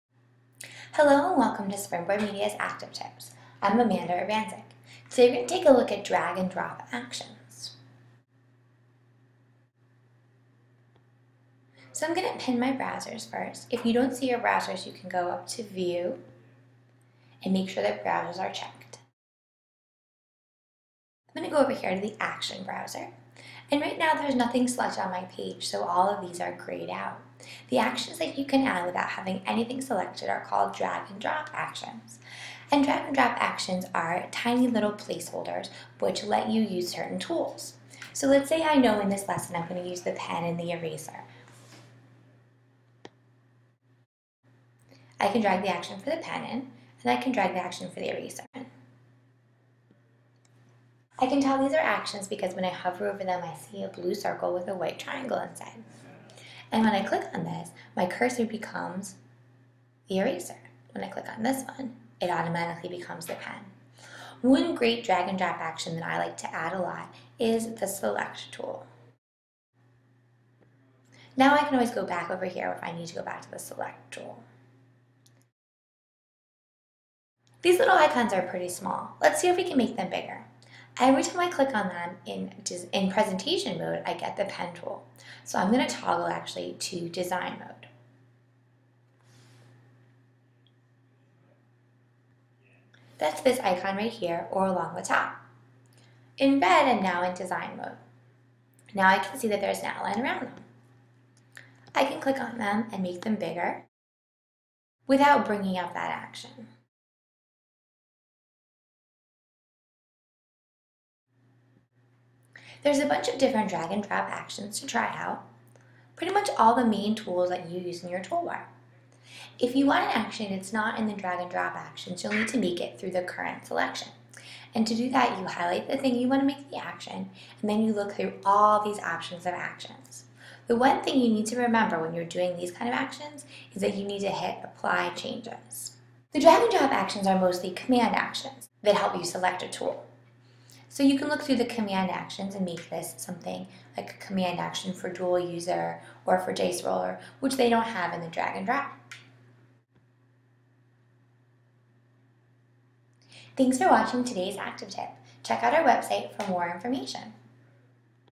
ActivTips - Adding Drag and Drop Actions to your Flipchart.m4v
How to add in Drag and Drop Actions which bring up tools. Drag and Drop actions are great if you need to switch between tools often in one lesson! Springboard Media offers Act 48 Accredited Professional Development for Educators - Check out